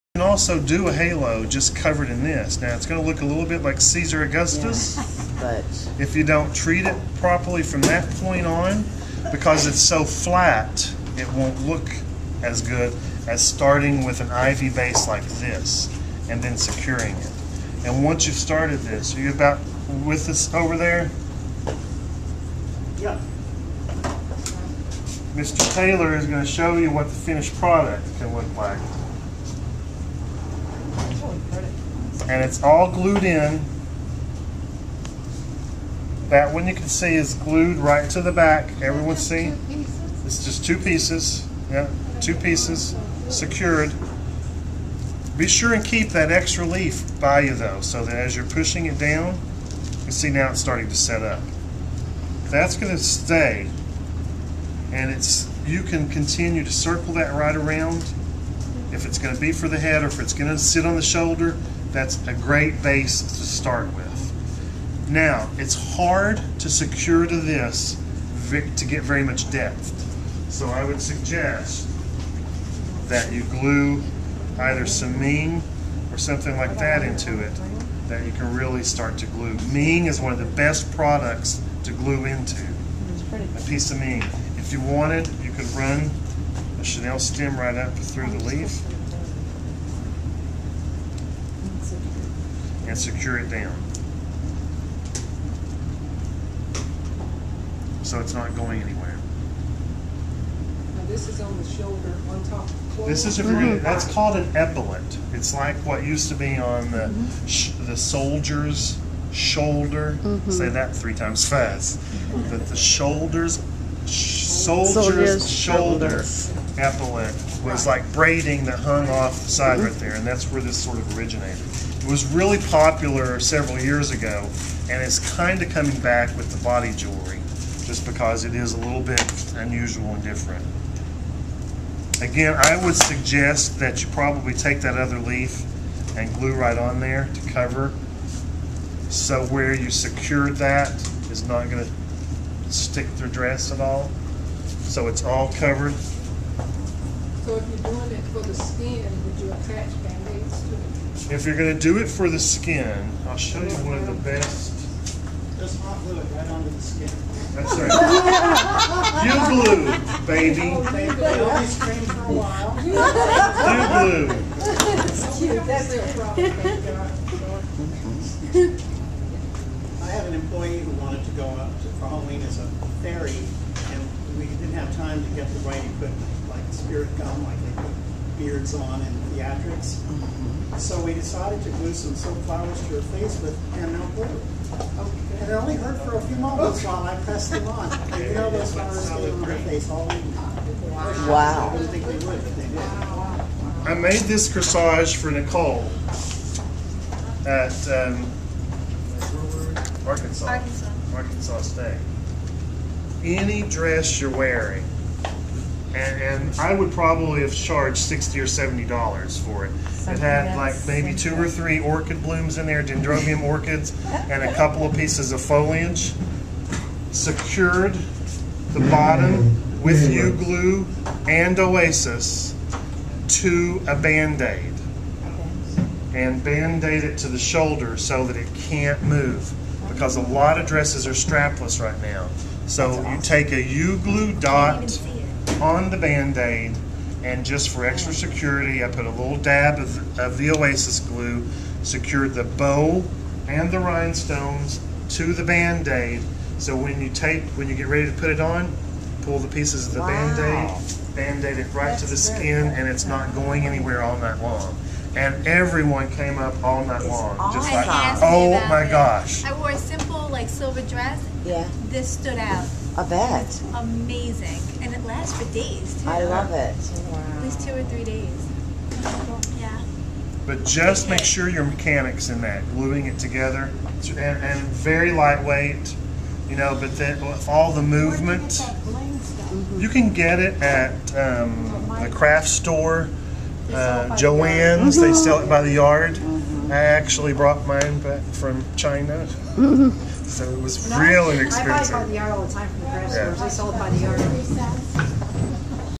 How to make a halo and a shoulder piece using U Glue
Jackie lacey demonstrates how to make a halo and a shoulder piece at the Floriology Institute during an advance Wedding Design Course.